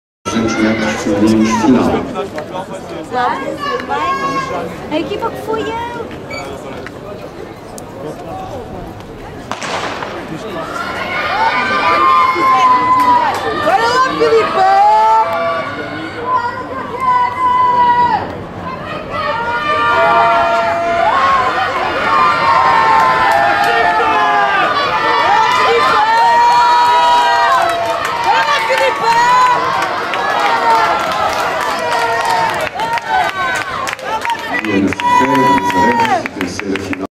2011-07-10 - CampNacJuniores (2Jorn) - 200m Fem Final.wmv
Campeonato Nacional de Juniores (2ª Jornada), 10 de Julho de 2011, Estádio Universitário de Lisboa
200m Fem Final  Vento: Desf: - 0,4
1ª Diana Cerqueira 93 Jun CAM-VC 24,49
2ª Filipa Martins 92 Jun SLB 24,98
3ª Cátia Jacinto 92 Jun GCAD 25,38
4ª Tatiana Rosário 94 Juv GDRCC 25,84
5ª Joana Vitorino 92 Jun CASM 26,00
6ª Sara Esteves 92 Jun SCB 26,04 (TM 26,044)
7ª Catia Azevedo 94 Juv SCP 26,04 (TM 26,046)
8ª Inês Bento 94 Juv CPP 26,40